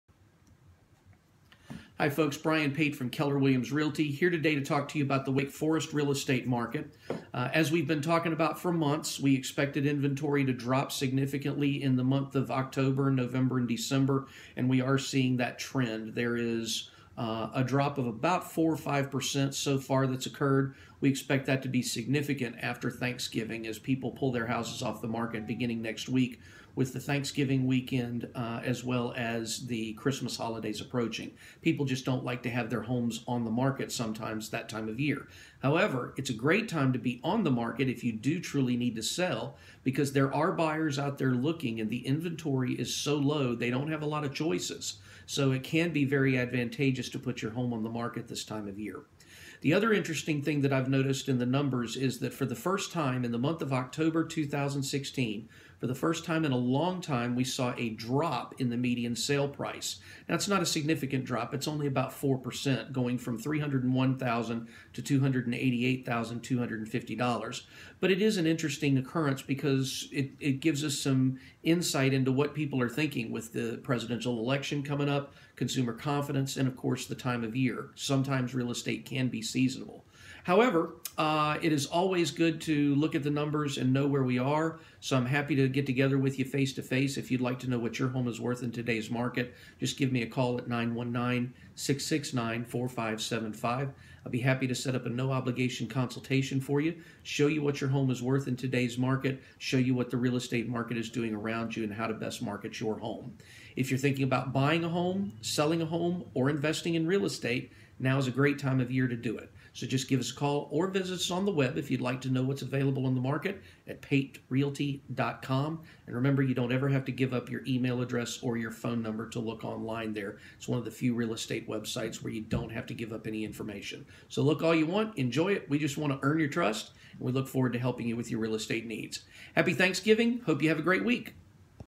Wake Forest, NC Real Estate
Wake Forest Realtor Brian Pate of Keller Williams Realty gives us an update on the most recent sold data in the 27587 zip code.